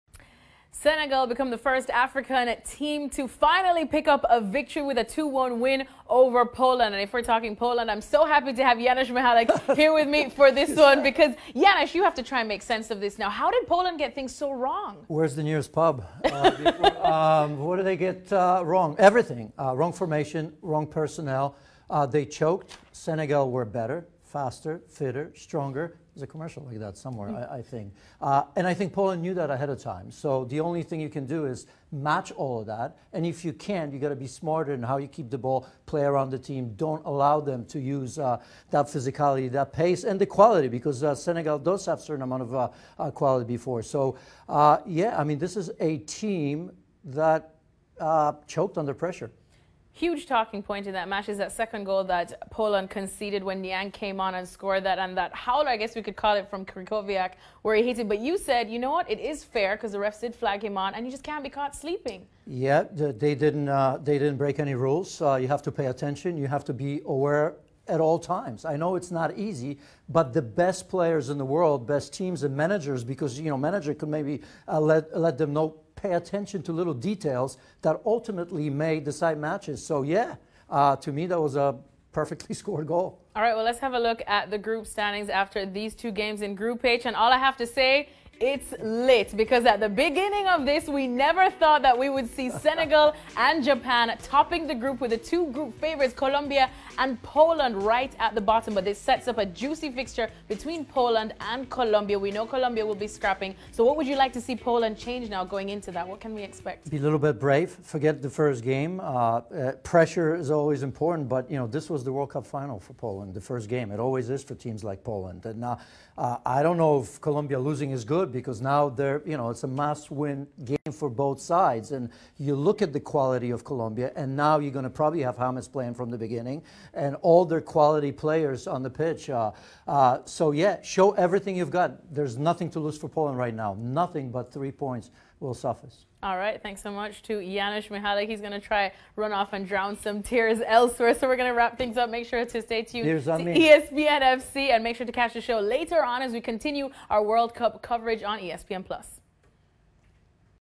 Poland 'choked' vs. Senegal in 2-1 loss at the 2018 World Cup | ESPN FC
ESPN FC's Janusz Michallik says Poland only have themselves to blame for mistakes in a 2-1 loss to Senegal.